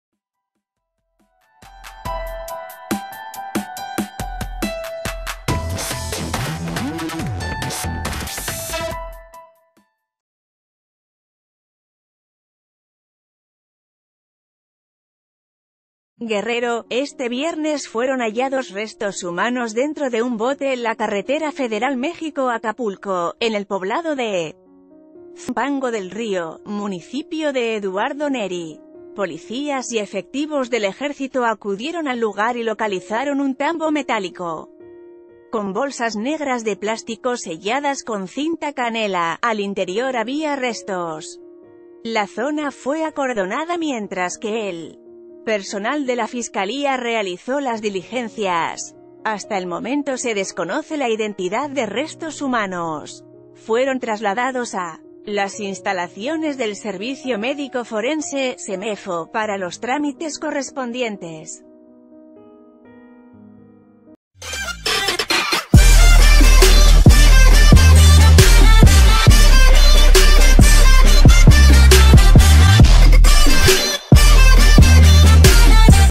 Abandonan restos humanos dentro de un bote en Zumpango del Río
Guerrero.- Este viernes fueron hallados restos humanos dentro de un bote en la carretera federal México-Acapulco, en el poblado de Zumpango del Río, municipio de Eduardo Neri. Policías y efectivos del ejército acudieron al lugar y localizaron un tambo metálico con bolsas negras de plástico selladas ...